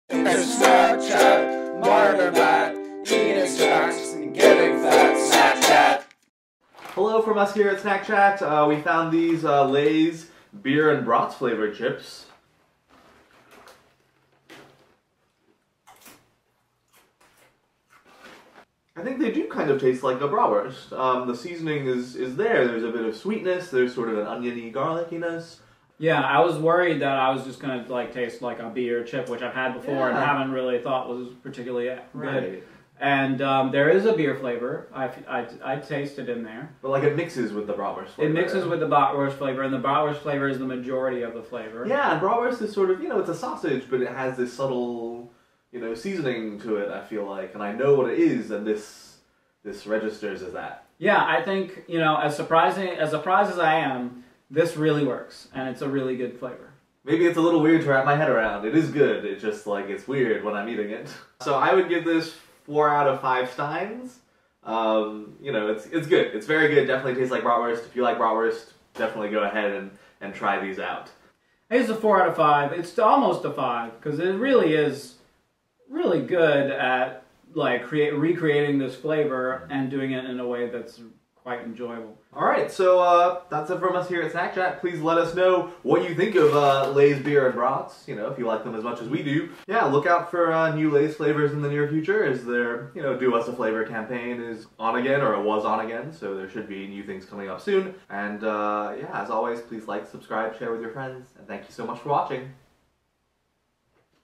Lay's Beer n' Brats
We thought Lay's would not be doing anything new until this years Do Us A Flavor contest came to the finals, but in the grocery store we saw Beer n' Brats flavord Lay's. So here we are with our thoughts.

We both gave them 4/5